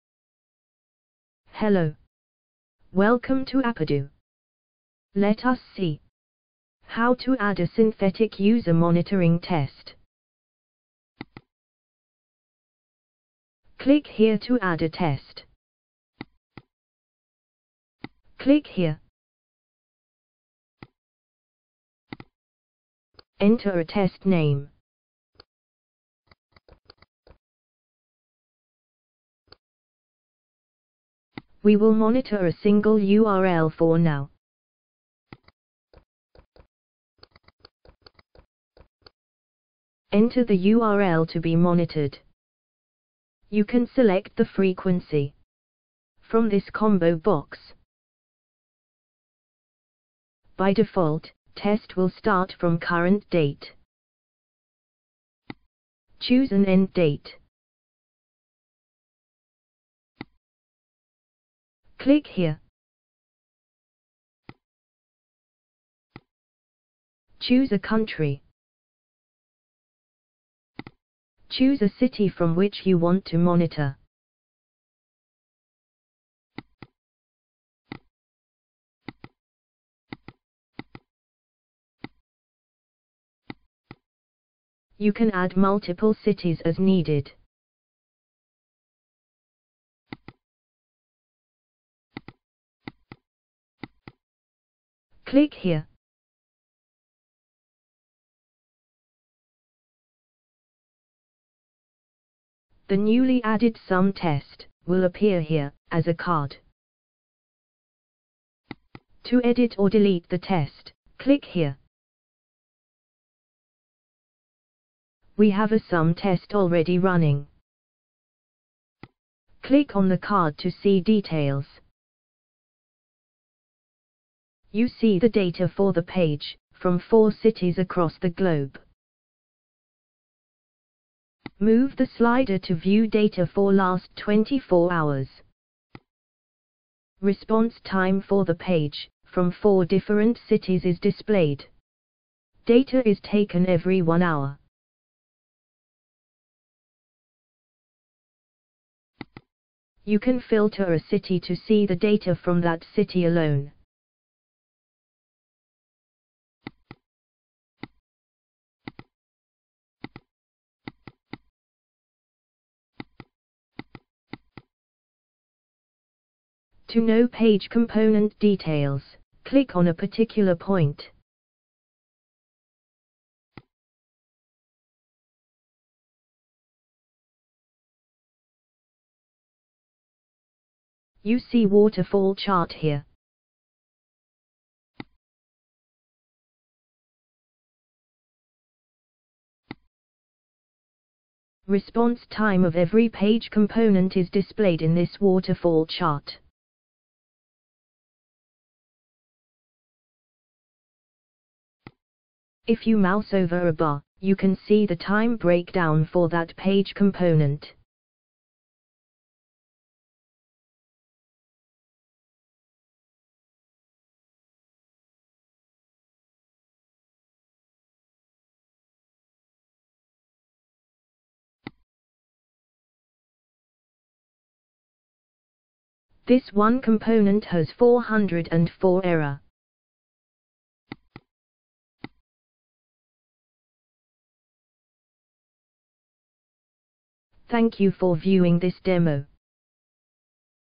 Appedo Synthetic User Monitoring
Monitor the speed of your web application from different cities across the globe.

Identify slow page components.